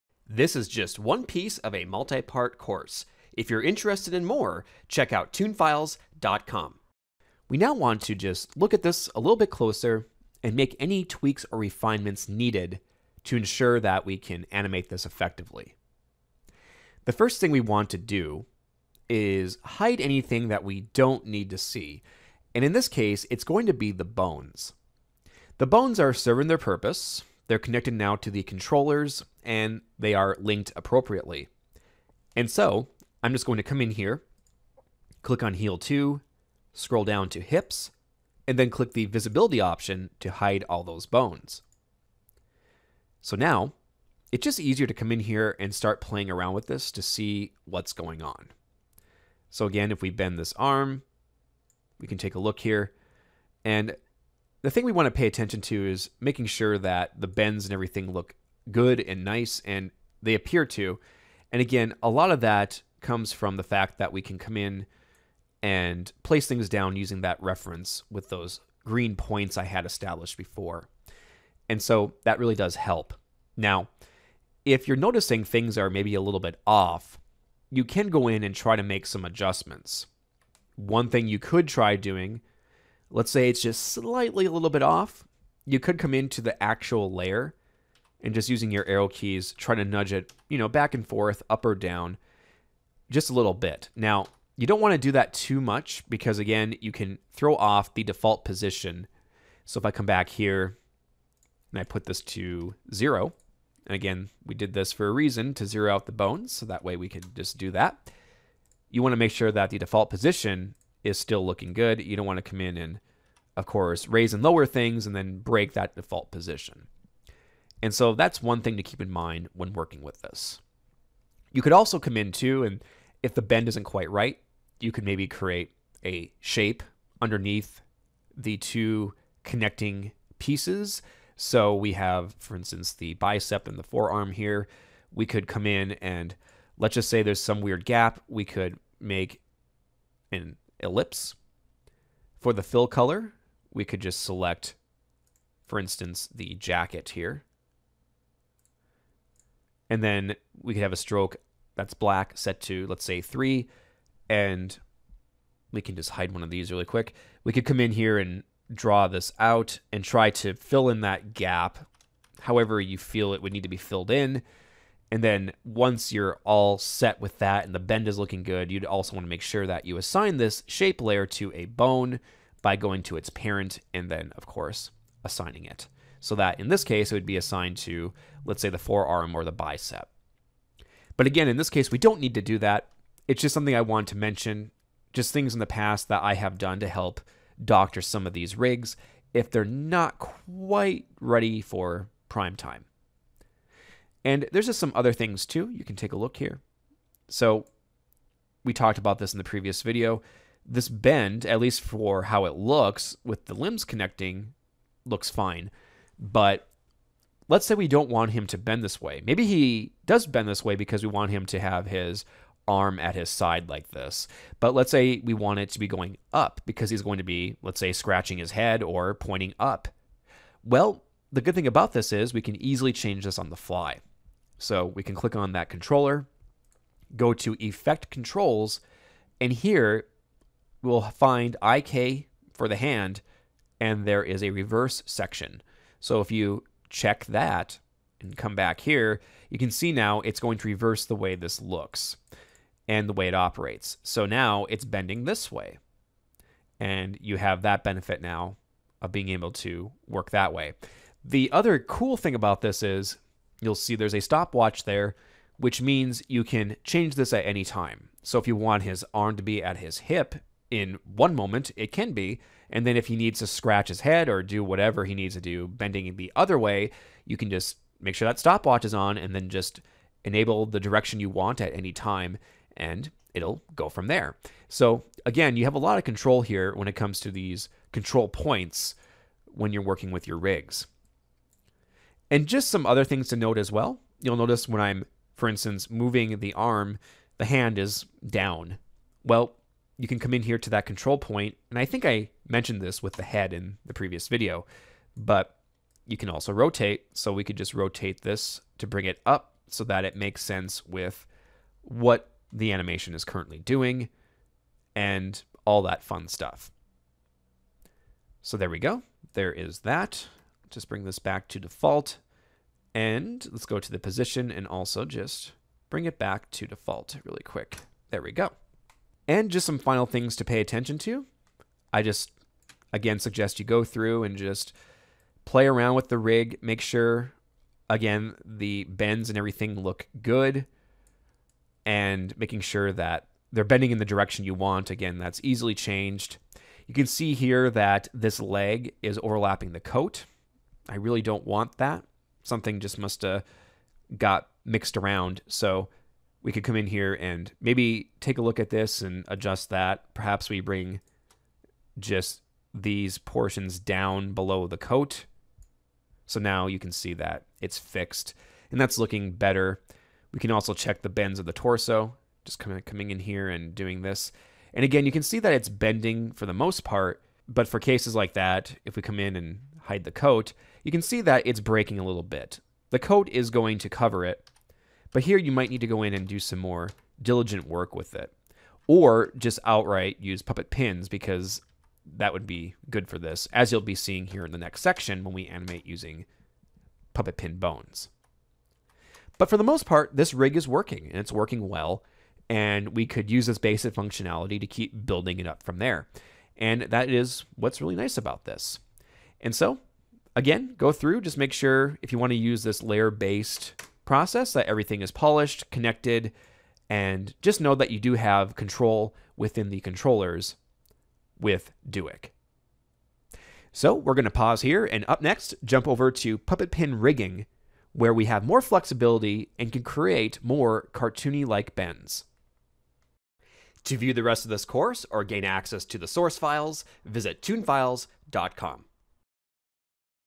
Refining Duik Motions in After Effects
You can easily refine Duik rigs by customizing some basic settings. In this video I go over a some of these settings you can apply for refinement.